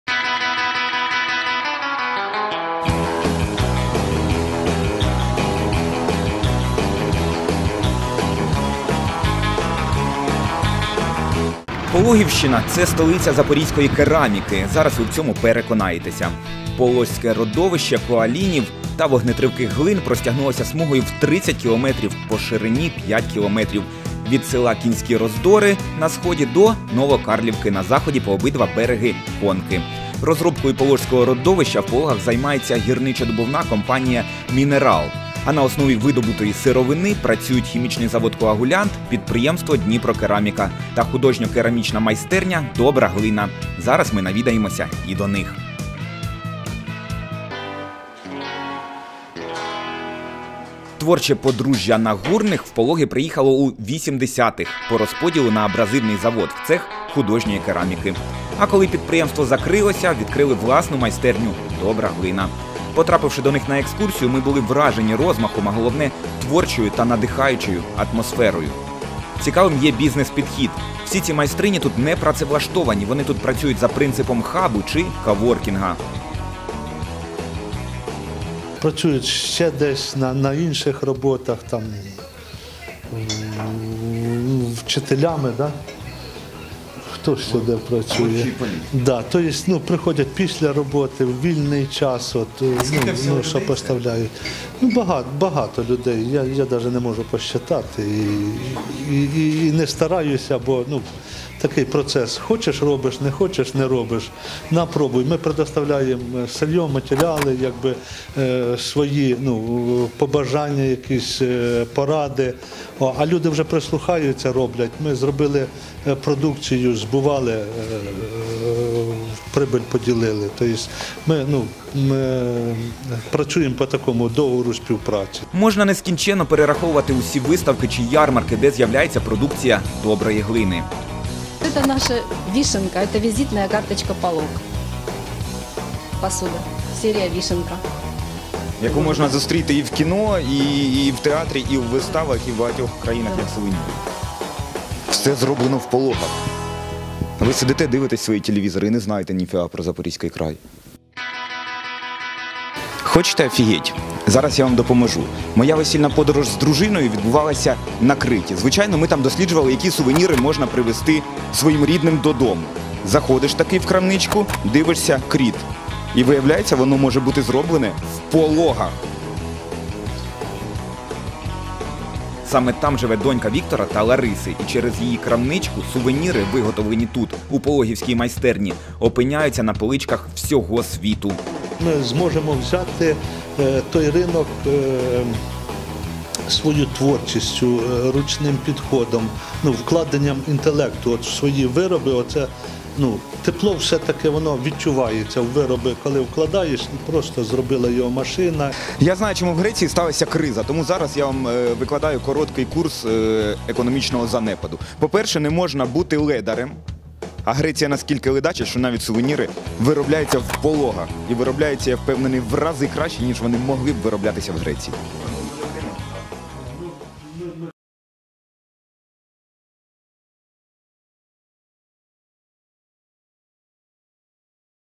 Керамічна майстерня "Добра глина"
В місті Пологи нам пощастило відшукати керамічну майстерню "Добра глина". Тут працюють за принципом хабу - нема найманих працівників. Всі майстрині приходять сюди у вільний від основної роботи час. Їм надаються всі інструменти і матеріали для виготовлення посуду. Прибуток від реалізованої продукції ділять навпіл із власником майстерні.
Більш того, в майстерні виготовляться сувеніри, які потім продаються в Греції. Як так вийшло - дивіться у сюжеті.